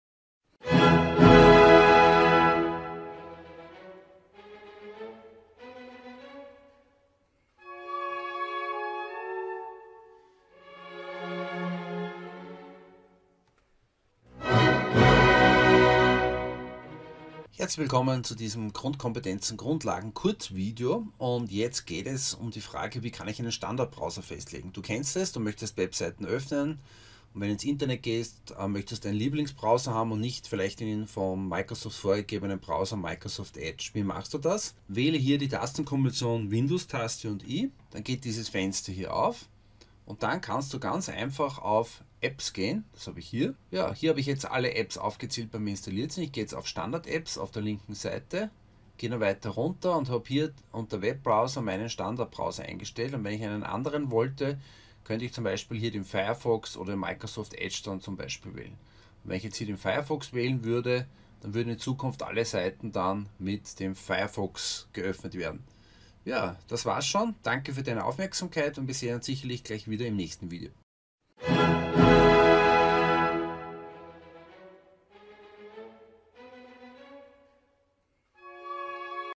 03 Wie kann man einen Standard - Browser festlegen?
Ich zeige hier, wie man festlegt, dass Webseiten und Internetinhalte mit einem bestimmten Browser - dem Standardbrowser - geöffnet werden. Dieses Video ist Teil meiner Playlist Windows 10 Grundkompetenzen / Grundlagen, die sich an Einsteiger*innen in das Betriebssystem richtet.

Hier ist der Link zur Playlist mit 46 Videos zum Thema Windows 10: